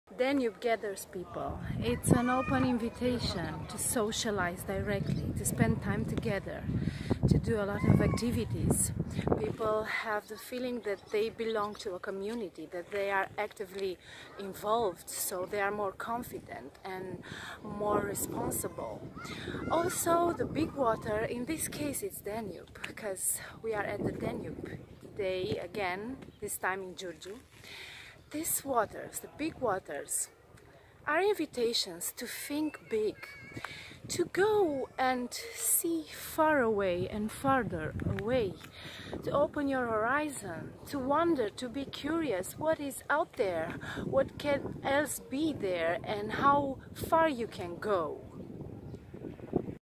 Shaped by Water | TEASER
FULL VIDEO verry SOON.
Living by the water beautifies you, makes you, shapes you in some surprizing ways. How come?